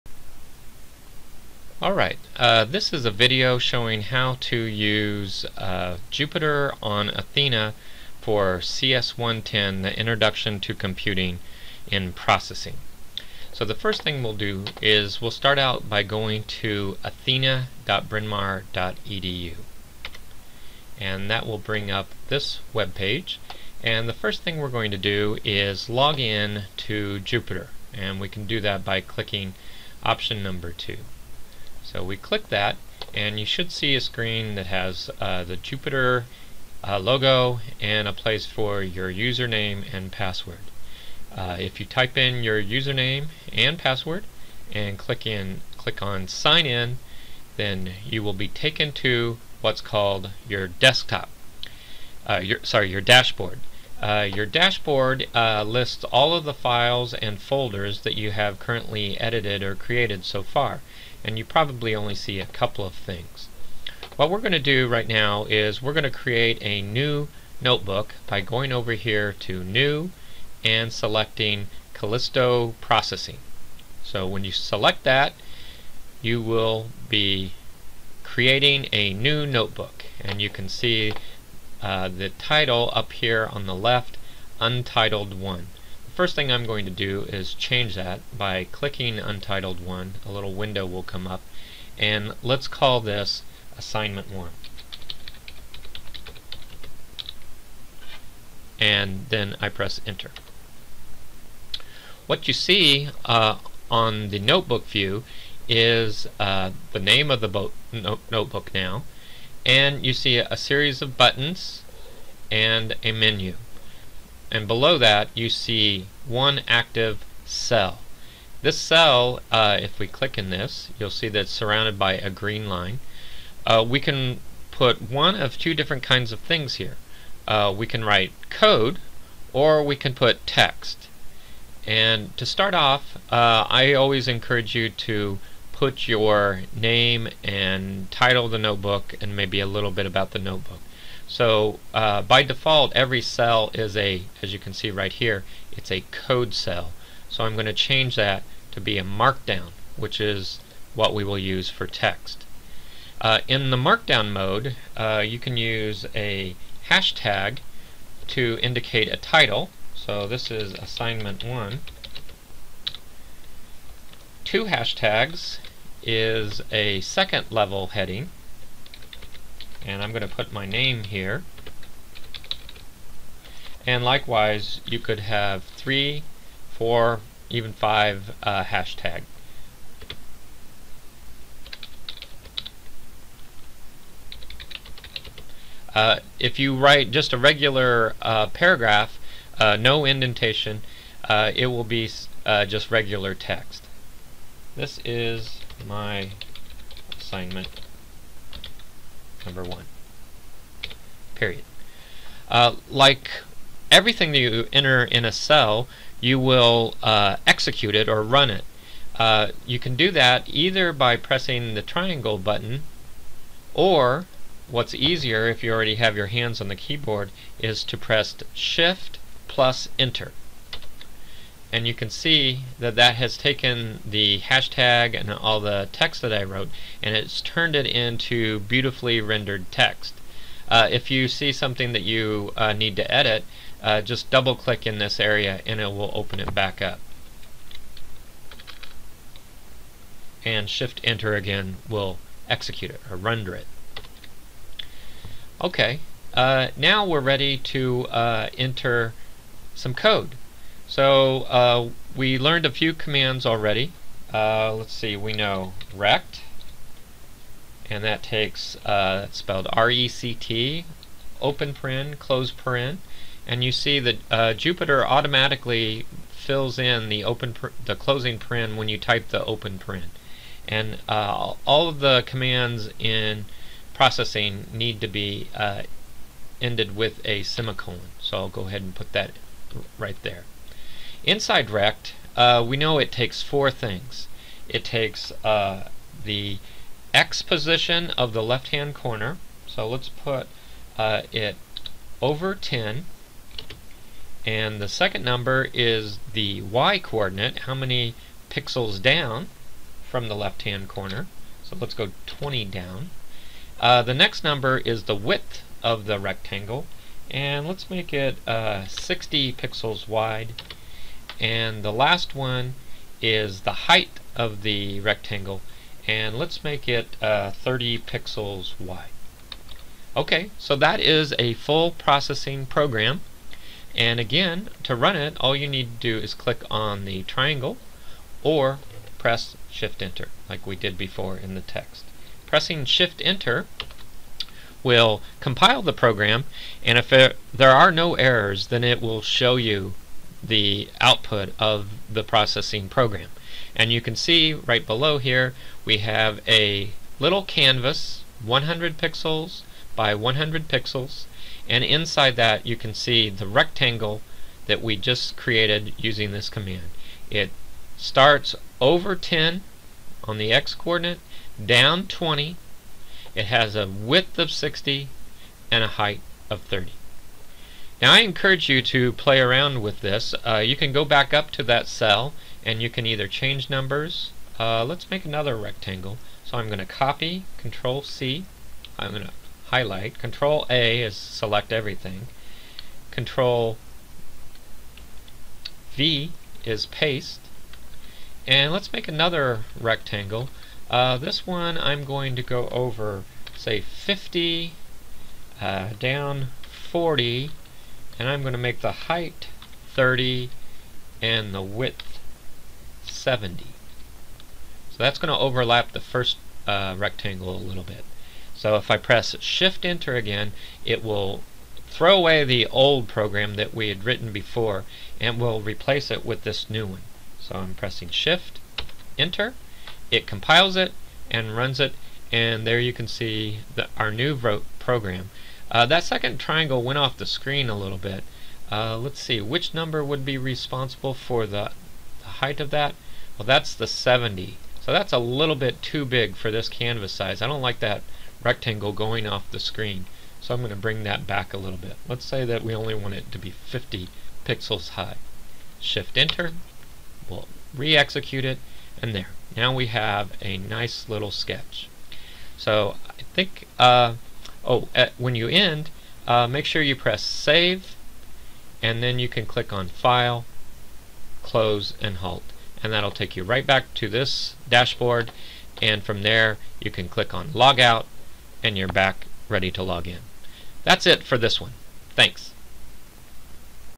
CS110: Getting Started with Processing on Jupyter
This video demonstrates using Processing with Jupyter on Athena at Bryn mawr College.